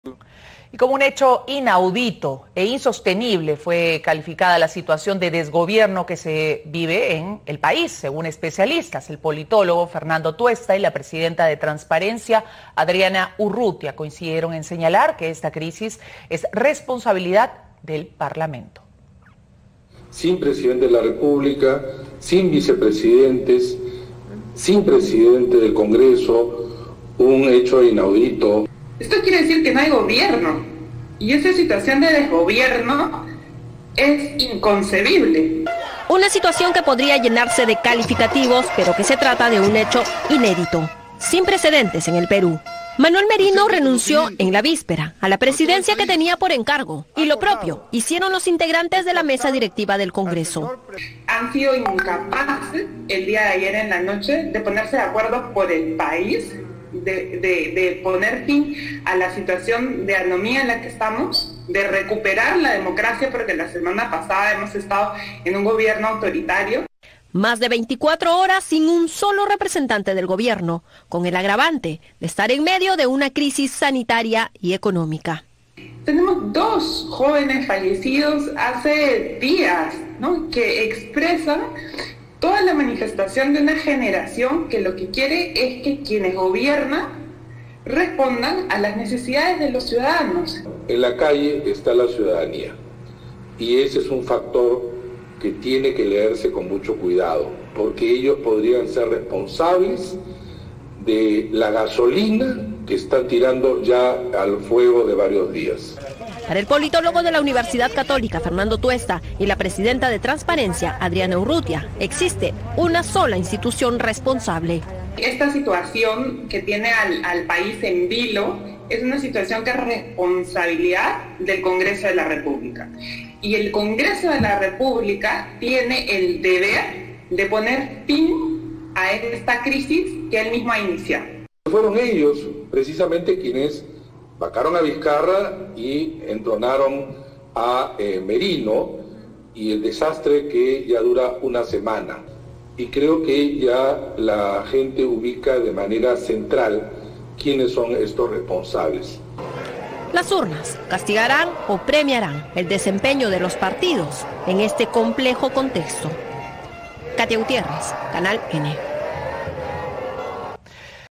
Incertidumbre y crisis en Perú
Manuel Merino renunció a la presidencia de la República y lo propio hicieron los integrantes de la Mesa Directiva del Congreso. Más de 24 horas sin representante del Gobierno. Para el politólogo de la PUCP, Fernando Tuesta y la Pdta. de la Asociación Civil Transparencia, Adriana Urrutia, existe una sola institución responsable: el Congreso.